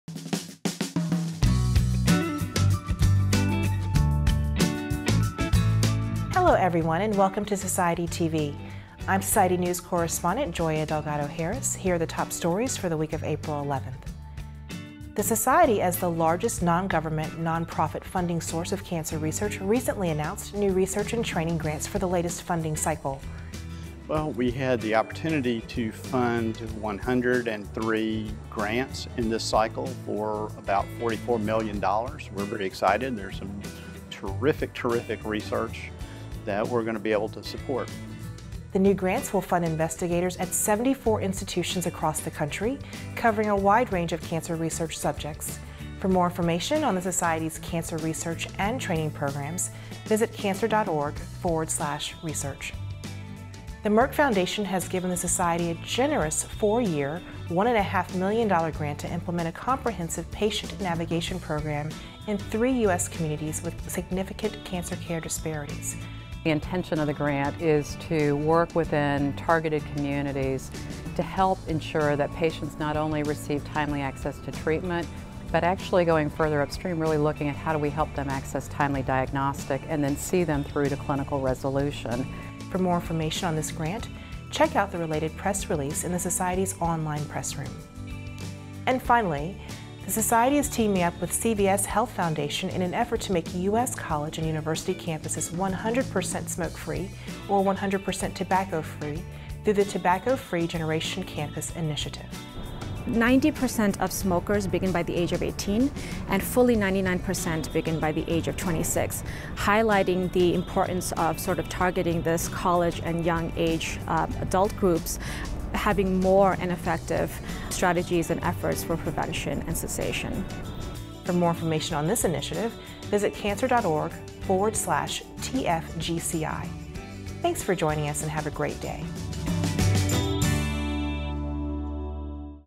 Society TV for early April
The Society funds over 100 new research grants this year, and how can millennials help create the first "cancer free generation"? These stories and more on this episode of Society TV.